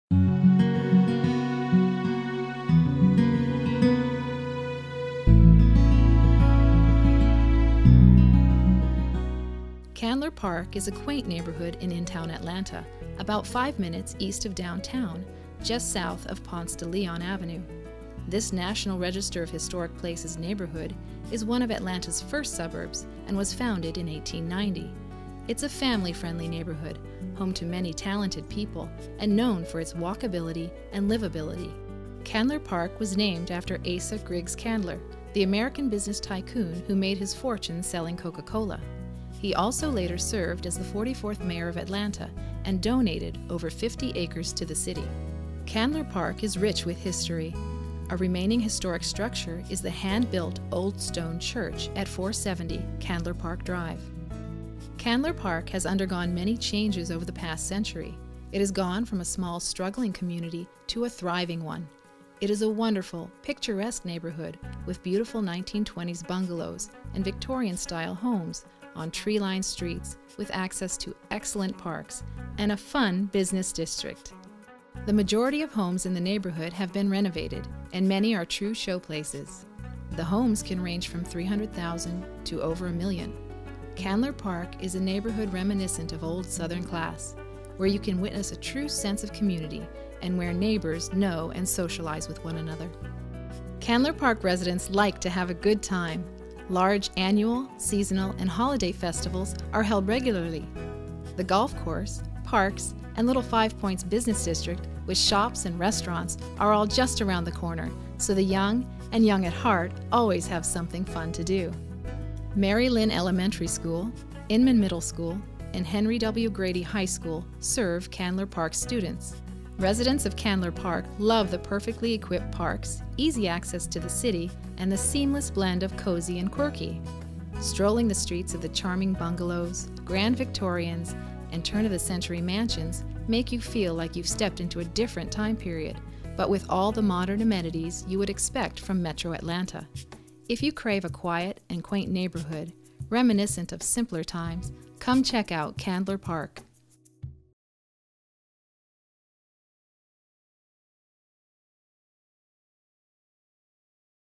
Life is Atlanta's Candler Park
Candler Park is a neighborhood in intown Atlanta, in zip code 30307, about 5 minutes east of downtown and just south of Ponce De Leon Avenue. This National Register of Historic Places neighborhood is one of Atlanta's first suburbs and was founded as Edgewood in 1890.

The neighborhood is home to many talented people, great shops, bars, and everything eclectic. It's a family-friendly neighborhood with a focus on walkability and livability.

The Candler Park Neighborhood Organization (CPNO) exists to support the neighborhoods interests, and has been around since 1971. CPNO bylaws are available here.

Here are some nice Candler Park photos, courtesy of JoJo Ross. Here is atlanta-midtown.com's Guide to Candler Park and here is the Wikipedia entry about Candler Park.

Candler Park Neighborhood Organization puts on an annual festival in Candler Park known as Fall Fest. This is popular festival attended by 10,000 people, and includes a great band line-up, artists' market, children's activities, 5K road race, tour of homes, and more. Fall Fest is the most recent in a long tradition of Candler Park festivals.

History
Candler Park has a rich and interesting past. Read about CPNO's history here. You can check out some old maps and images in this Flickr gallery.

Candler Park has undergone many changes over the past century, and aspects of its history have been forgotten along the way. Thanks to recent research by the Early Edgewood-Candler Park BiRacial History Project, we can now add another dimension to the Candler Park of decades past: including the rich history of its African American working class neighborhood, and probing local racial dynamics of the 100+ years since the neighborhood was founded.